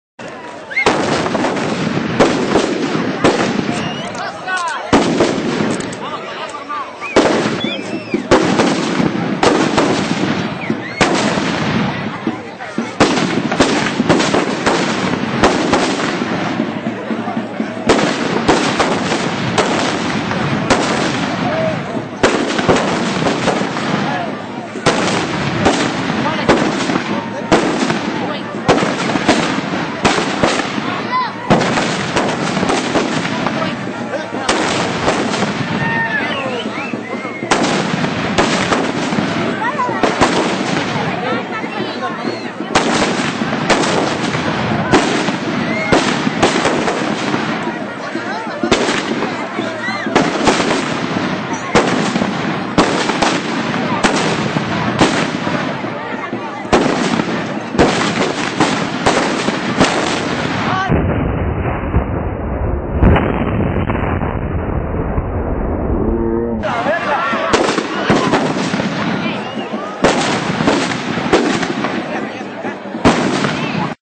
Hilarious Exploding Mallet Accident
Silvester oder so im anderen Land kp aufjedenfall wurde beim letzten übertrieben mit schwarzpulver oder was die da halt benutzen